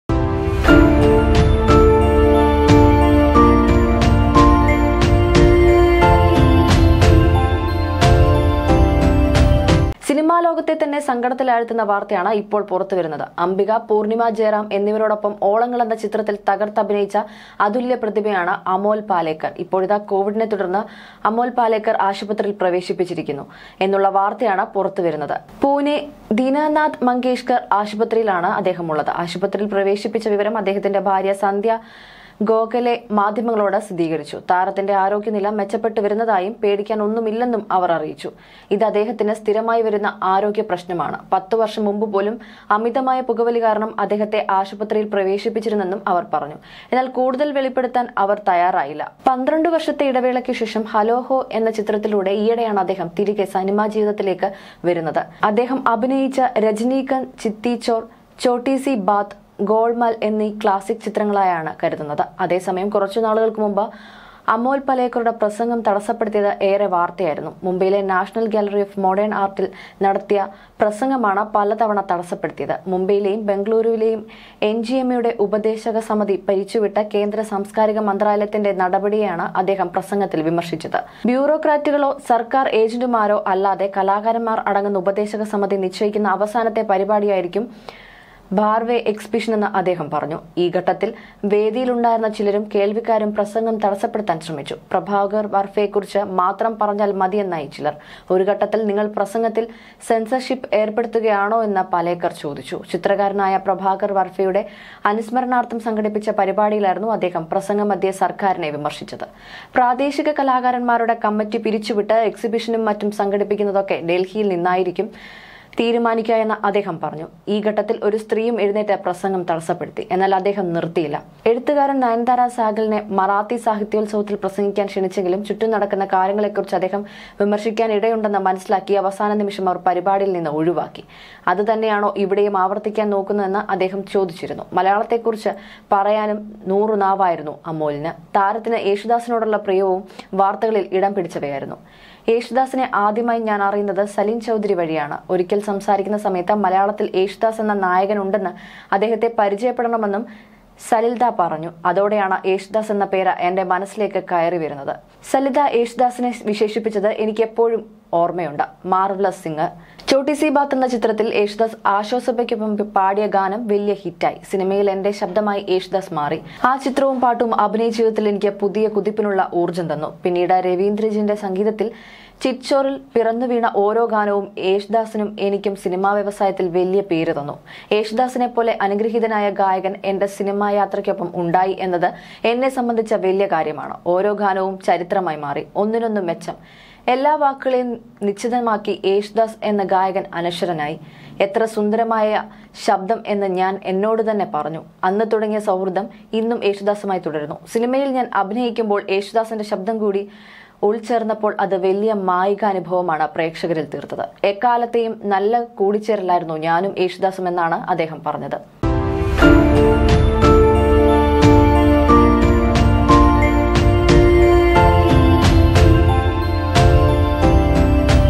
Actor | എങ്ങും പ്രാര്‍ത്ഥന മാത്രം
അതേസമയം, കുറച്ച് നാളുകള്‍ക്ക് മുമ്പ് അമോല്‍ പലേക്കറുടെ പ്രസംഗം തടസ്സപ്പെടുത്തിയ് ഏറെ വാര്‍ത്തയായിരുന്നു. മുംബൈയിലെ നാഷണല്‍ ഗാലറി ഓഫ് മോഡേണ്‍ ആര്‍ട്ടില്‍ നടത്തിയ പ്രസംഗമാണ് പല തവണ തടസ്സപ്പെടുത്തിയത്. മുംബൈയിലേയും ബെംഗളൂരുവിലേയും എന്‍ജിഎംഎയുടെ ഉപദേശക സമിതി പിരിച്ചുവിട്ട കേന്ദ്ര സാംസ്‌കാരിക മന്ത്രാലയത്തിന്റെ നടപടിയേയാണ് അദ്ദേഹം പ്രസംഗത്തില്‍ വിമര്‍ശിച്ചത്...@MovieBrand794